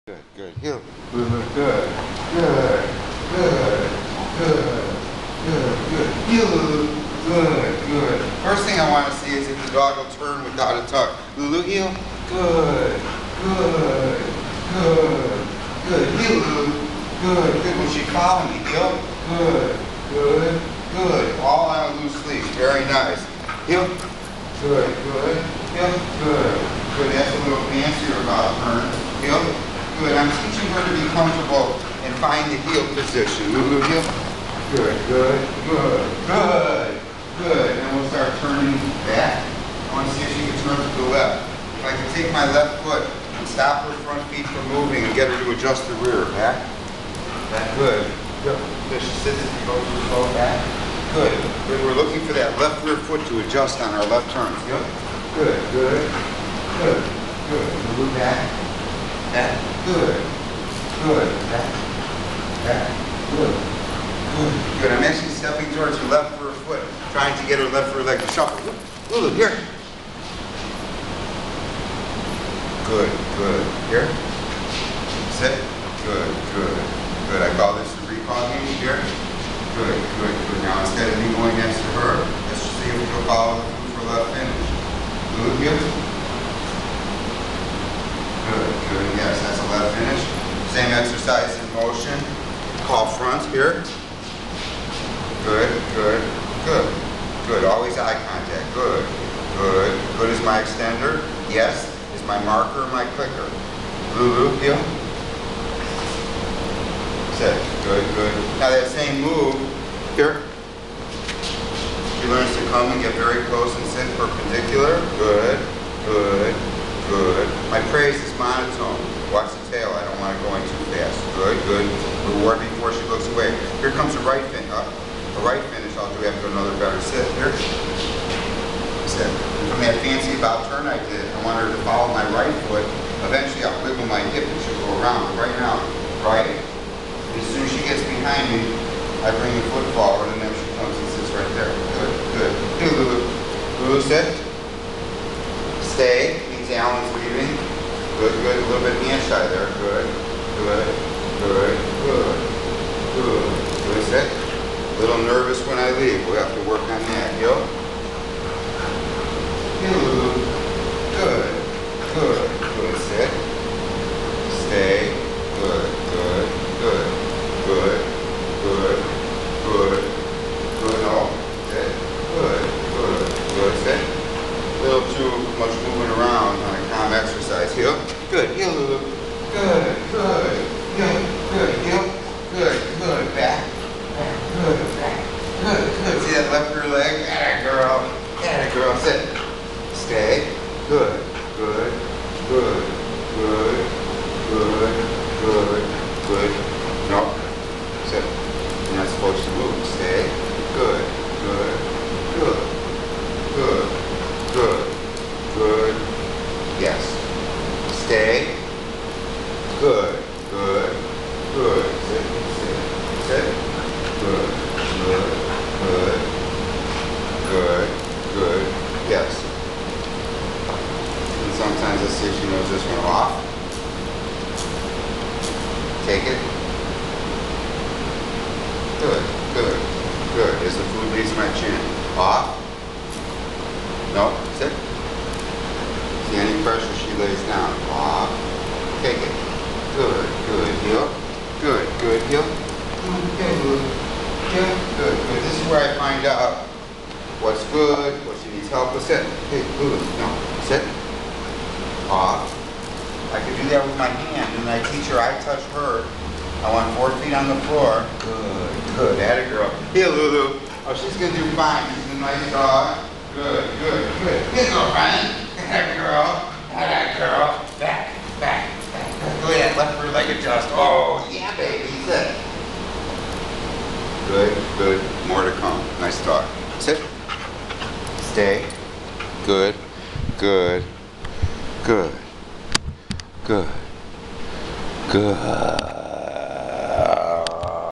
Allan Ross Dog Training
Lulu B. is a 2.9 year old Labrador Retriever in her first session of her first Board and Train.  Here we practiced as I find what obedience she knows in the Canine College.  I begin as we bond and I earn her trust.  She is a nice dog and will do well.